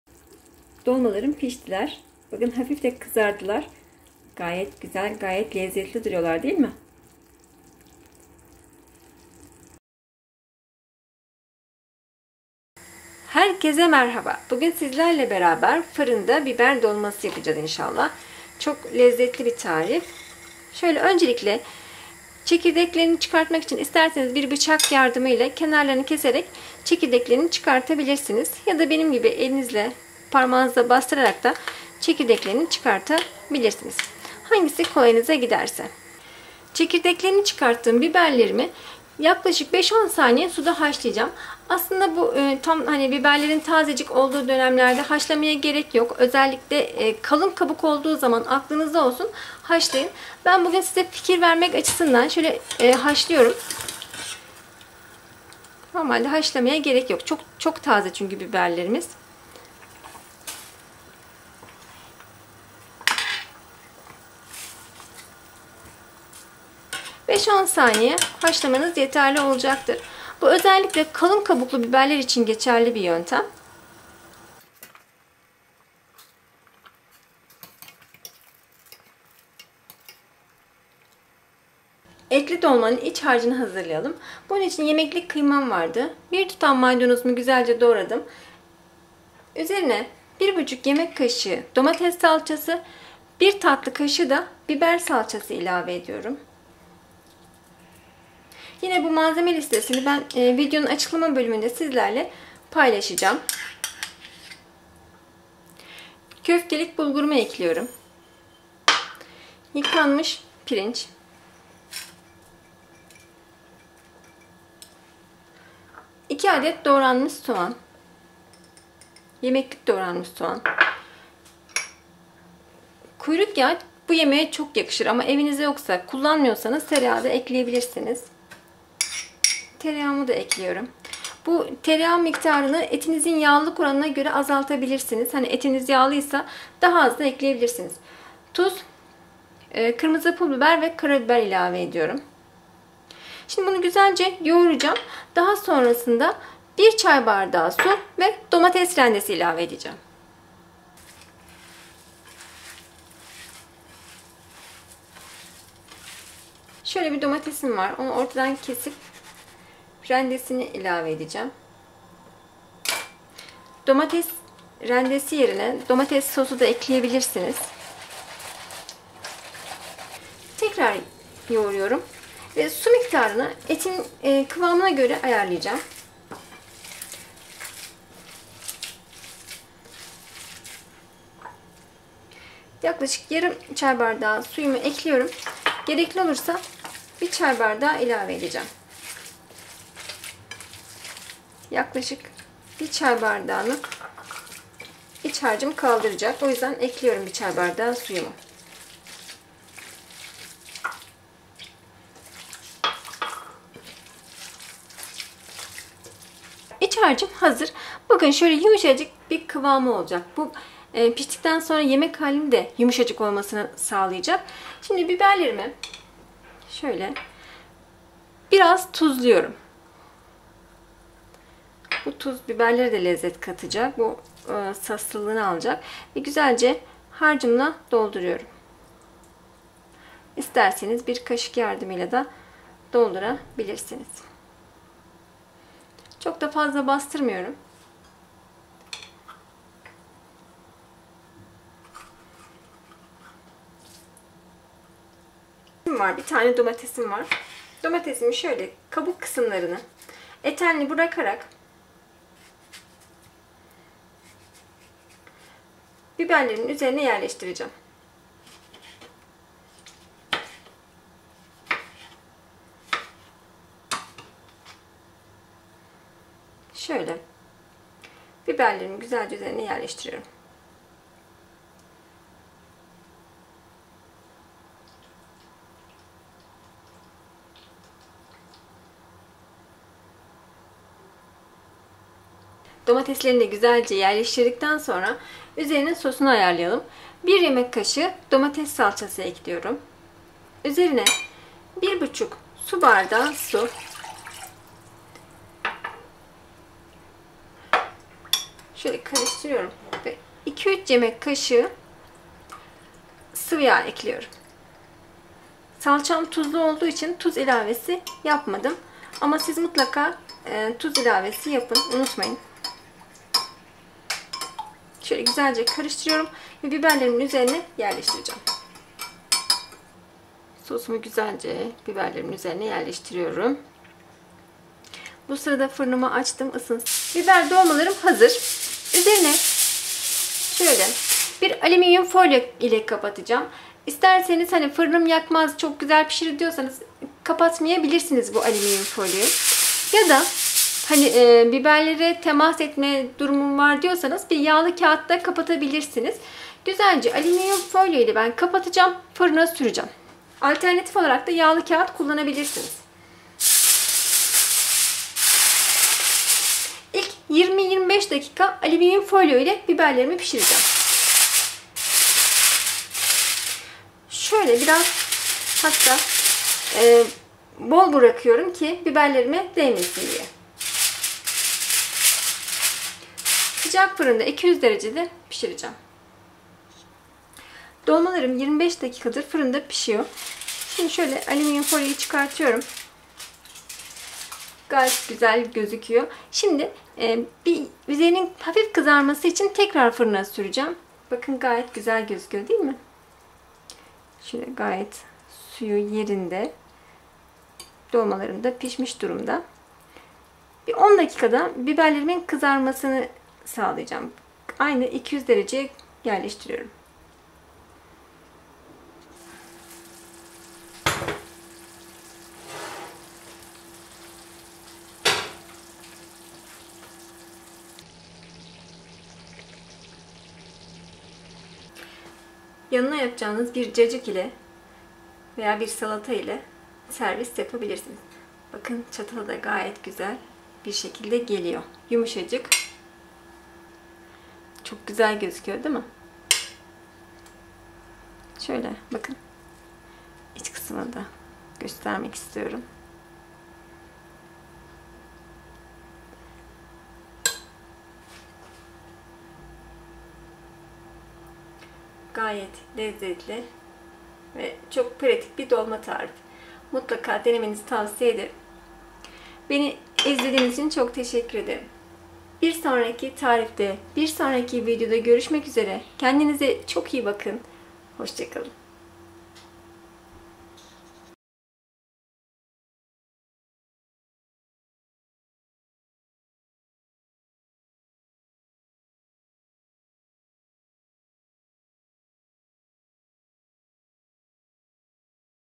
FIRINDA ETLİ DOLMA
Yemelere doyamayacağınız lokum tadında etli biber dolması tarifi. Bu tarifi kaçırmayın. Mutlaka deneyin.
Farklı tarifler ve püf noktaları için

FIRINDA ETLİ BİBER DOLMASI İÇİN GEREKLİ OLAN MALZEMELER

• Yaklaşık 20-25 biber dolması kullanılıyor. Dolma biberlerin küçük veya büyük olması sebebiyle tam bir sayı vermek istemiyorum.

• Tercihe göre domates veya patlıcan da ekleyebilirsiniz.

• 300_400 gr yemeklik kıyma

• Eğer kullanacağınız et kaburga olursa yemeğiniz lezzetli olacaktır.

• 1-2 adet orta boy soğan

• 1 çay bardağı köftelik bulgur

• 1 çay bardağına yakın yıkanmış pirinç
** Tercihe göre pirinç miktarını azaltabilirsiniz.

• 1 tutam maydanoz

• 1.5 yemek kaşığı domates salçası

• 1 tatlı kaşığı biber salçası

• 1 tatlı kaşığı kırmızı pulbiber

• 1 çay kaşığı karabiber

• 1 tatlı kaşığı tuz

** Baharatları kendi zevkinize göre azaltabilir veya çoğaltabilirsiniz.

• 2 yemek kaşığı tereyağı

**Eğer kullanıyorsanız tereyağı yerine kuyruk yağı kullanabilirsiniz.

• Eti sulandırmak için 1 çay bardağı su

• 1 domates rendesi

** Eğer dolma için domates kullanacaksanız, domatesin içini çıkarıp, doğrayıp, kıymalı iç harcına ekleyebilirsiniz.

SOSU İÇİN GEREKLİ OLAN MALZEMELER

• 2-3 yemek kaşığı sıvıyağ

• 1 yemek kaşığı domates salçası

• 1,5 su bardağı su

• Gerekli görürseniz tuz